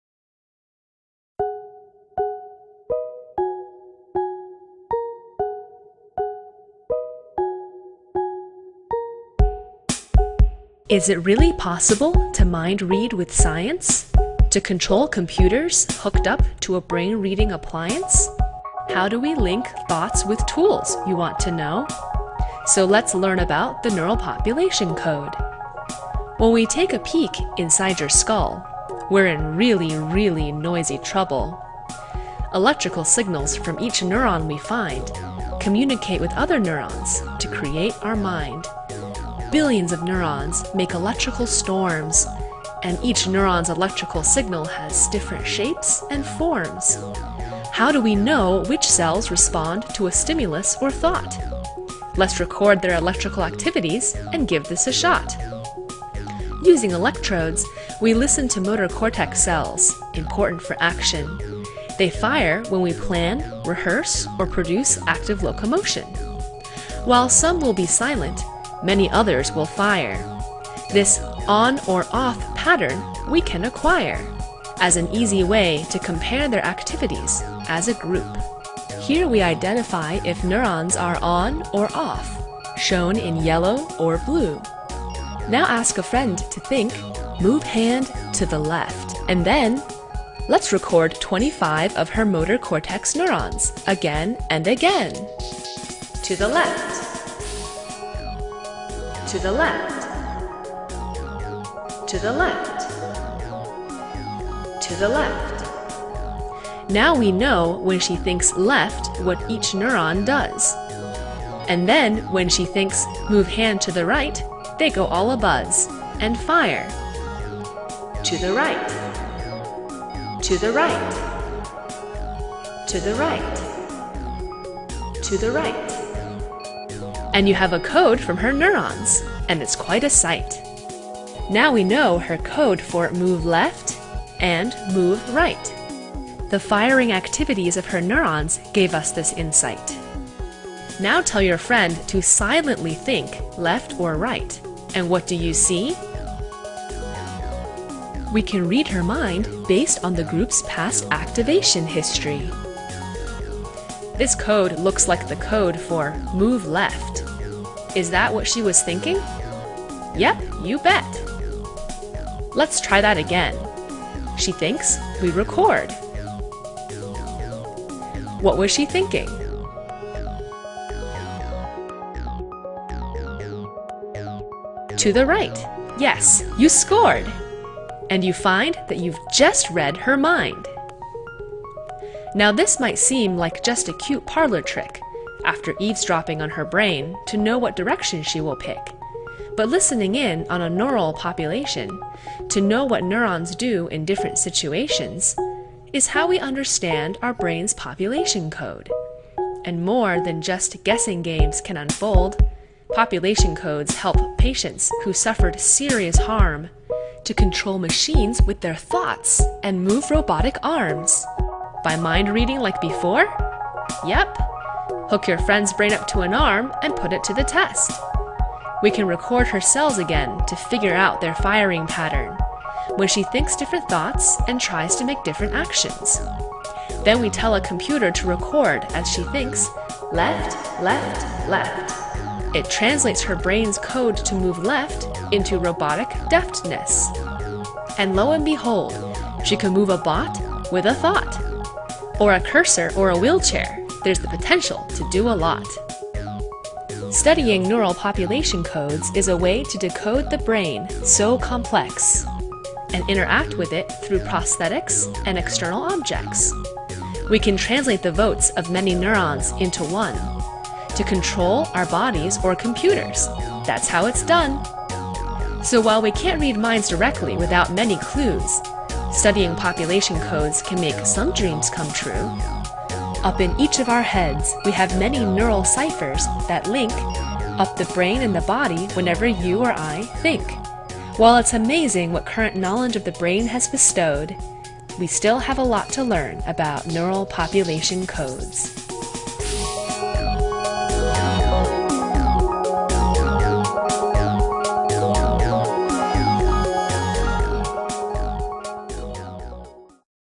Brain Awareness Video Contest: Population Coding: Mind Reading and More
Is it possible to read minds using science? While scientists cannot read thoughts, they are able to anticipate more basic neural responses through a process called population coding. Watch this video by Vania Cao, who recently defended her PhD thesis at Brown University, to learn about population coding. Discover how scientists use this process to “read” minds and make bionic limbs move. This video tied for second place in the 2013 Brain Awareness Video Contest.